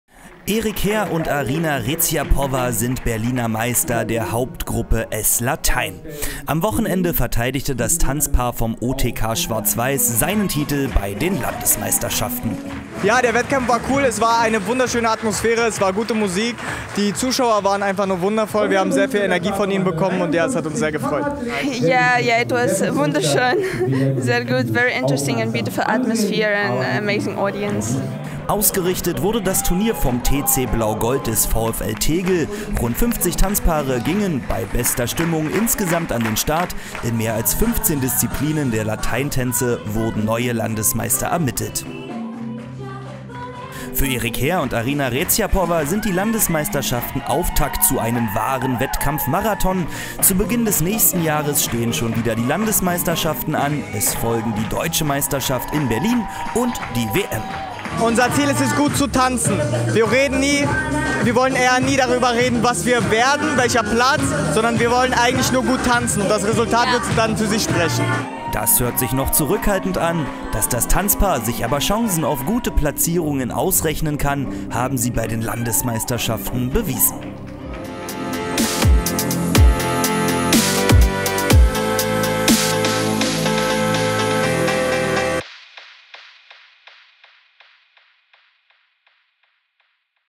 Berliner Meisterschaft Lateintanz 2021
Erik Heer und Arina Reziapova sind Berliner Meister der Hauptgruppe S Latein. Am Wochenende verteidigte das Tanzpaar vom OTK Schwarz-Weiß seinen Titel bei den Landesmeisterschaften. Ausgerichtet wurde das Turnier vom TC Blau Gold des VfL Tegel. Rund 50 Tanzpaare gingen - bei bester Stimmung - insgesamt an den Start. In mehr als 15 Disziplinen der Lateintänze wurden neue Landesmeister ermittelt. Für Erik Heer und  Arina Reziapova sind die Landesmeisterschaften Auftakt zu einem wahren Wettkampf-Marathon. Zu Beginn des nächsten Jahres stehen schon wieder Landesmeisterschaften an, es folgen die Deutsche Meisterschaft in Berlin und die WM.

sportfanat.de - das Videoportal für Berliner Sport und Sportvereine

sportfanat.de ist ein Angebot von Fensterplatz-Film